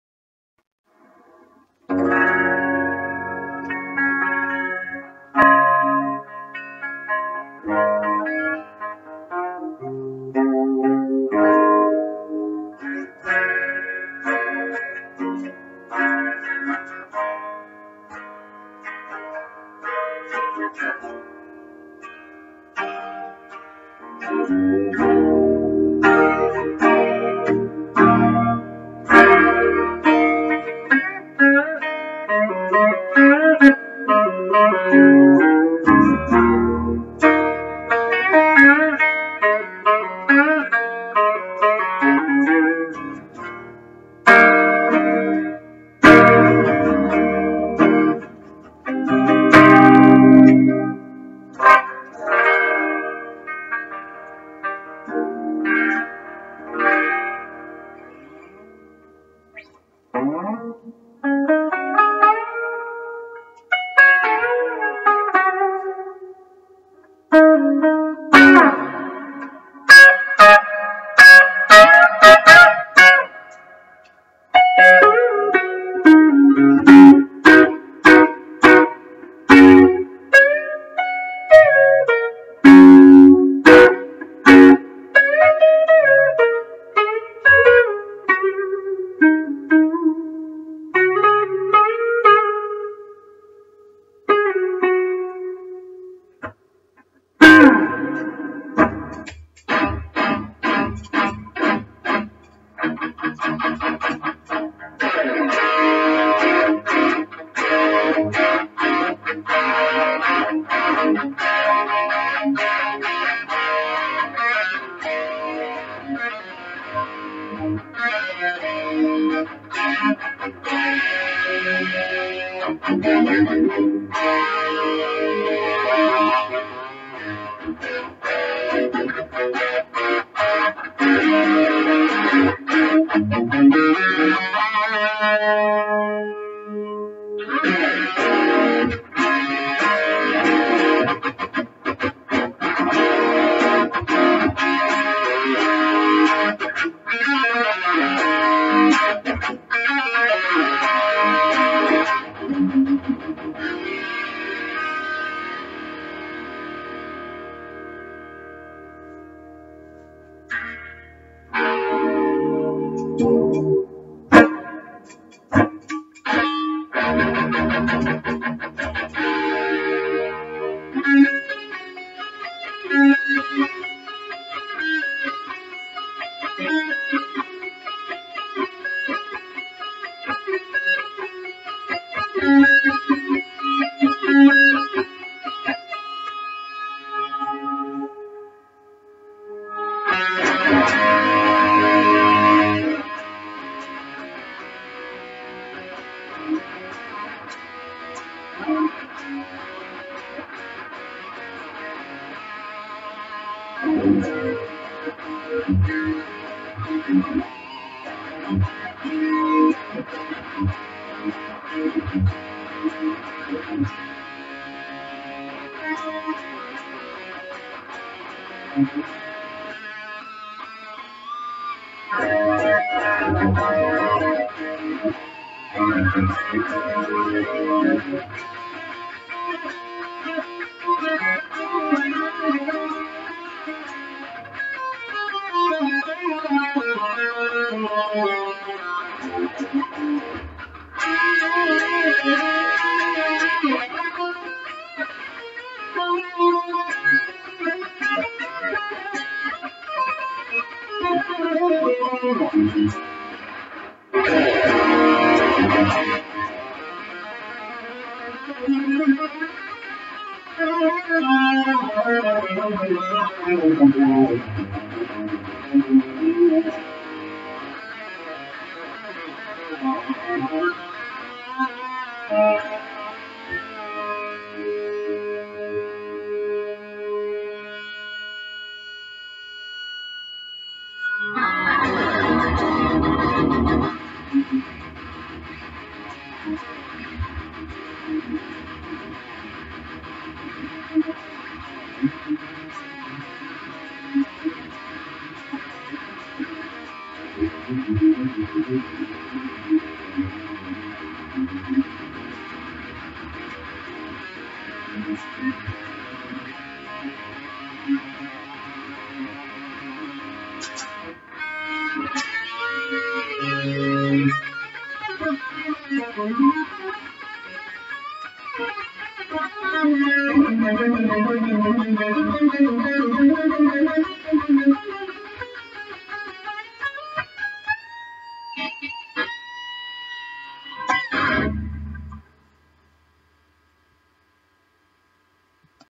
quick demo 1959/2203/#36
I recorded this on a laptop and the sound quality is not great at times so the true tone is not always present.

Quick sample of my 3 channel amp mod. Just completed it last night. All 3 channels footswitchable. The 1959 channel has its own designated gain pot to dial in sparkly cleans, or add a bit of grit and crunch. This channel has a 2nd master volume in series so I can balance the volume jump between the 1959 mode and the other 2 modes.I also have a "muting"circuit in this mode so to avoid channel bleed.  In 2203 mode the circuit is slightly modified in order to compliment #36 mode. Nothing to drastic though. #36 mode adds another gain stage to the 2203 mode.  The video is just me screwing around and improvising as I go. There are a few GNR tunes in the #36 mode. At the end I adjust the eq in #36 mode for some scooped thrash metal! lol.

From 0 to 1:45 is "1959 mode. 1:45 to 2:45 is 2203 mode. The rest is #36 mode. I eq it at the end for some scooped metal!!! lol.